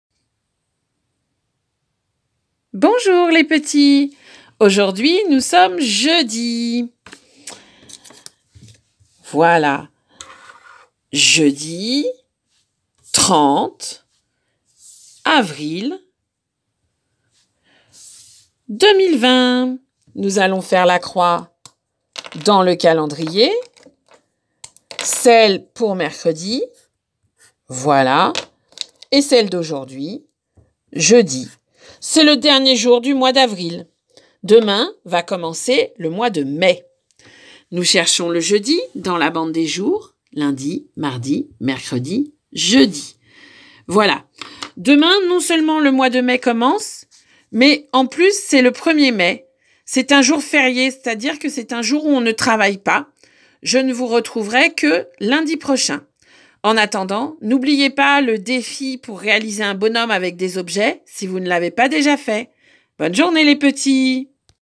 jour 3 semaine 5 petite section
petit rituel de la date en petite section pour l'école à la maison